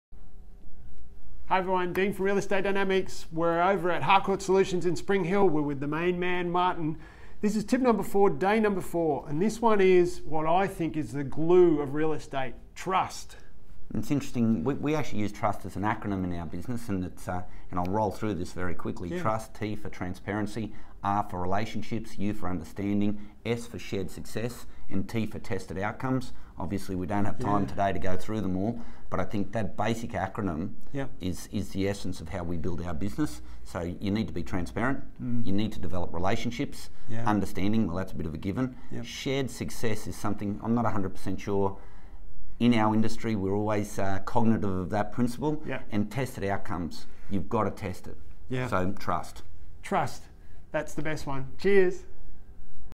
“What are the 5 things that develop TRUST” Quick Tip #4 Martin Millard CEO Harcourts Solutions
“What are the 5 things that develop TRUST” Quick Tip #4

Martin Millard CEO Harcourts Solutions believes there is 5 core things that help you develop TRUST in Real Estate.